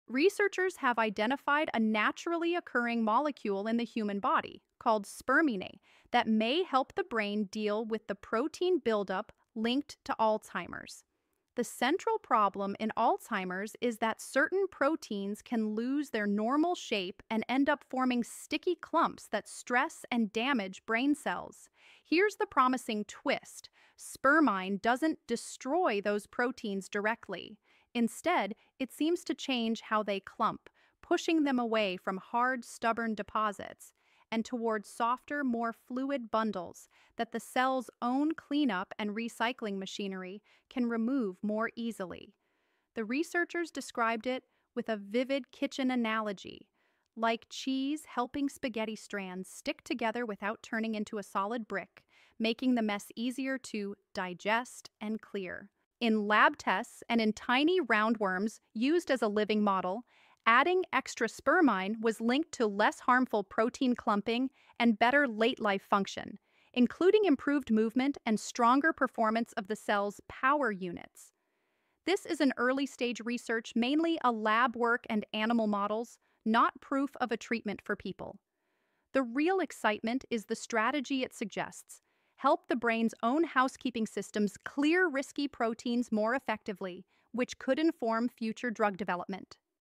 Spermine Eats Away Alzheimer’s Clumps
Scientists at Paul Scherrer Institute (PSI) discovered that a naturally-occurring molecule, Spermine, can help the body clear harmful brain proteins linked to Alzheimer's disease and Parkinson's disease. When extra spermine was given to worm models with Alzheimer’s- or Parkinson’s-like pathology, the animals maintained healthier cells longer. In lab tests, spermine caused misfolded proteins (like tau protein and alpha‑synuclein) to condense into droplet-like structures that cells can dispose of more easily via autophagy — effectively “melting” toxic protein clumps so the brain’s waste-removal system can handle them. While the findings are early and in non-human models, they point to a promising, natural path toward new therapies for neurodegenerative diseases.